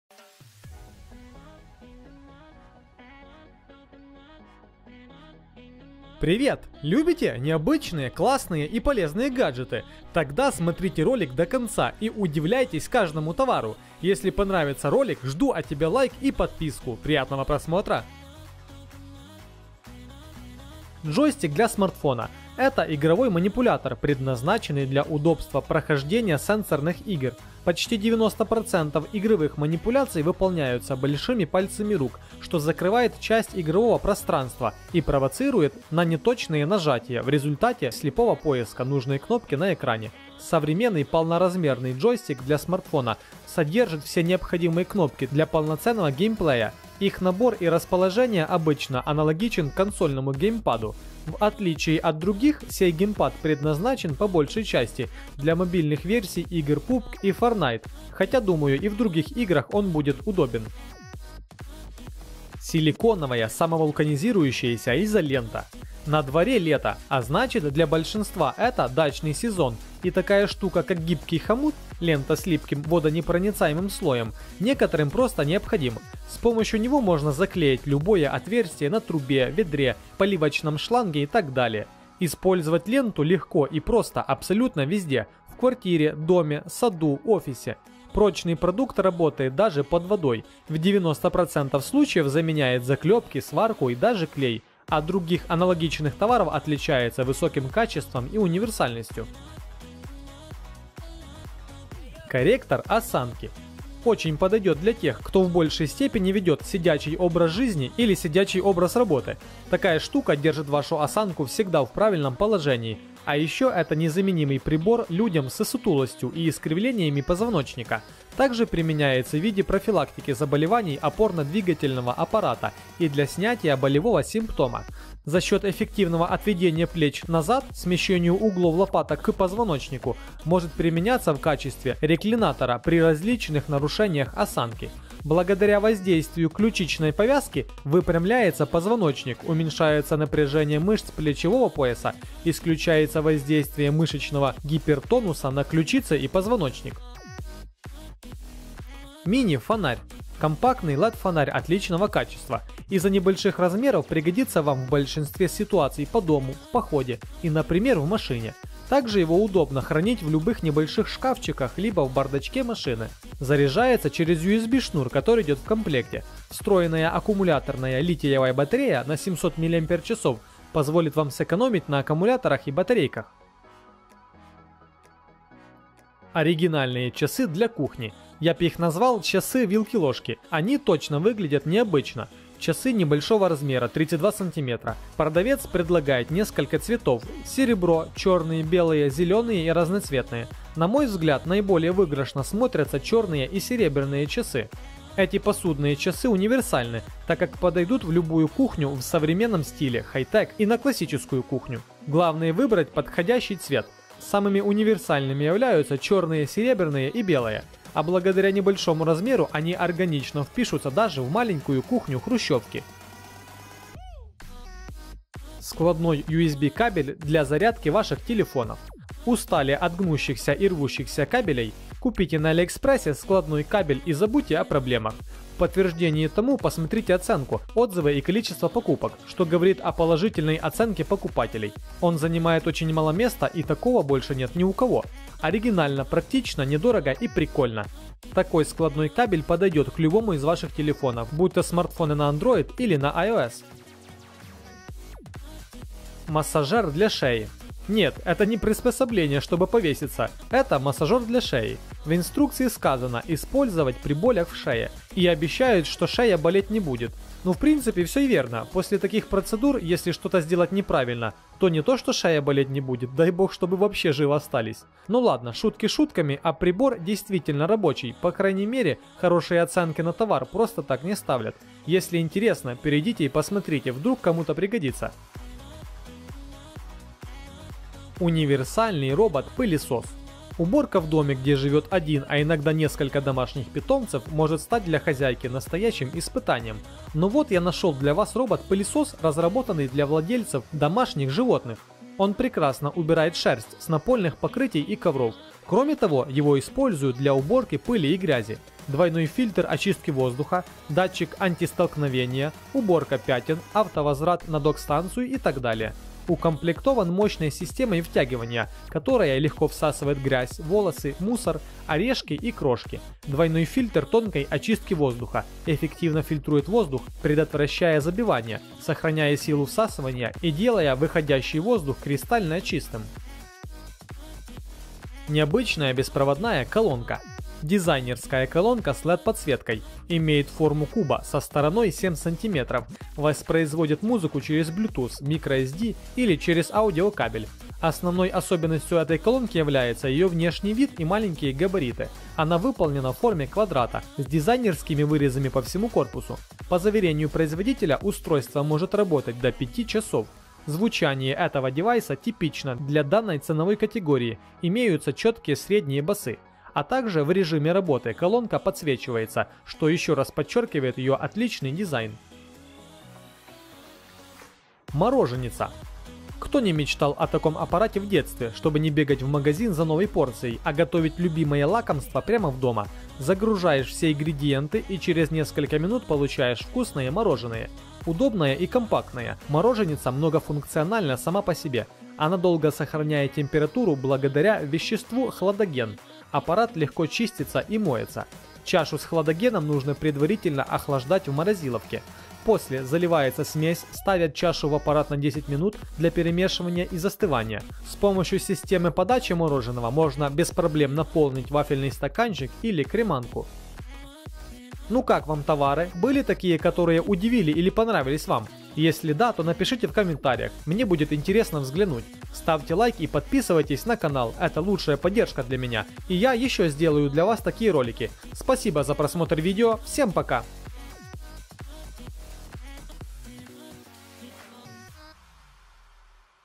ВО КИТАЙЦЫ ДАЮТ! ТОВАРЫ НА ЛЮБОЙ СЛУЧАЙ ИЗ ЖИЗНИ С ALIEXPRESS
5. ОРИГИНАЛЬНЫЕ ЧАСЫ ДЛЯ КУХНИ

6. СКЛАДНОЙ КАБЕЛЬ для зарядки телефона